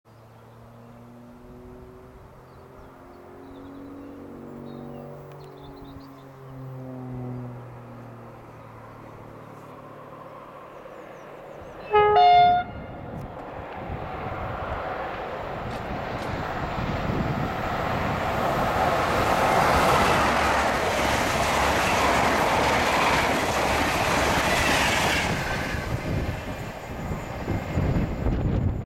East Coast 90
90026 Heads south with DVT 82302 on the rear with 1Z88 a special from Newcastle Central to Kings Cross. Passing Anglers Crossing Offord Cambs at 1143 on Sunday 9th May 2010. And the sun was out!